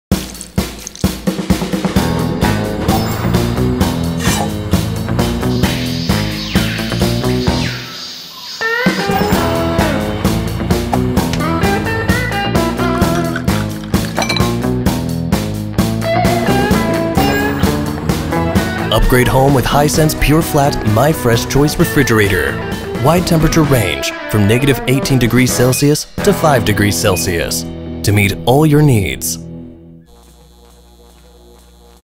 Hisense Refrigerator Series | My Fresh Choice
🎵Bounce to the beat of My Fresh Choice

The My Fresh Choice Zone can be easily converted from a fridge and freezer with -18℃~5℃ temperature setting. By creating the perfect temperature environment you need, you can efficiently prolong the freshness and taste quality of your desired food.